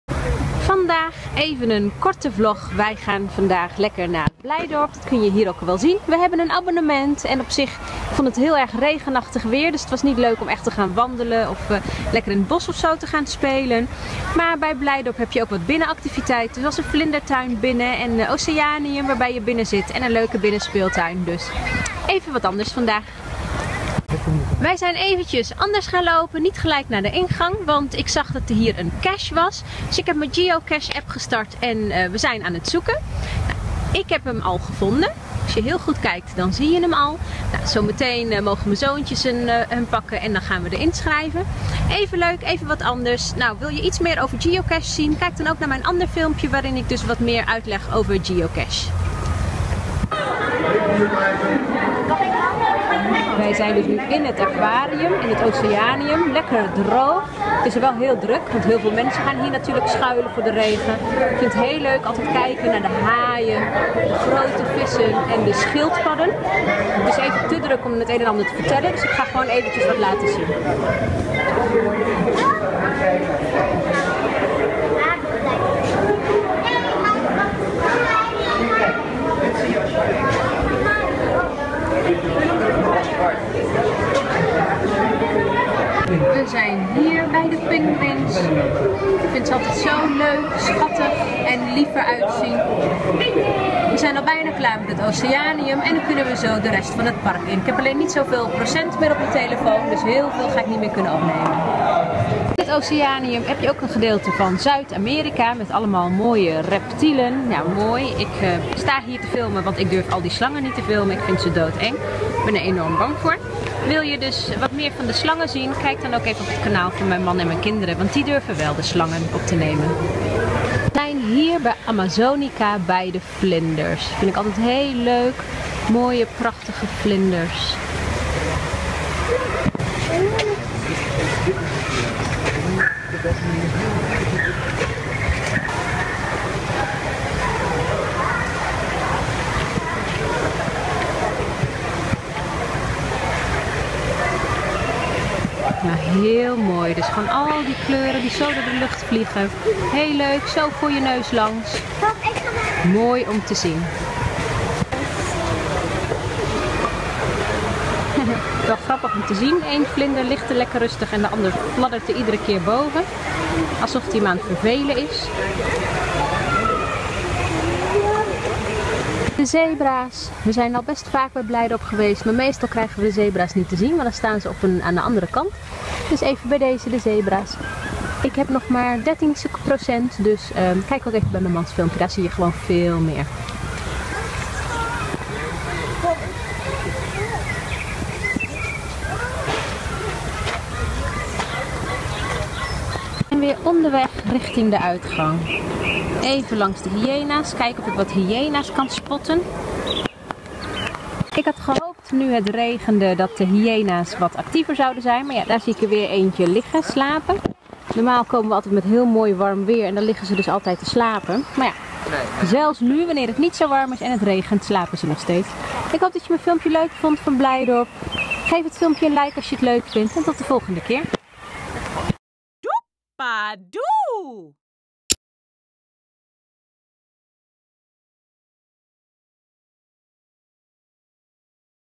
Diergaarde Blijdorp Rotterdam Zoo
Ik ben gisteren met mijn kinderen op stap gegaan naar Blijdorp in Rotterdam. Ik vind het een hele leuke en mooie dierentuin. Leuk om met mijn kinderen naar toe te gaan.
Wil je mijn nieuwste filmpjes niet missen?

Volg me op Twitter: @JufJannieApps.

Wil je meer ideeën?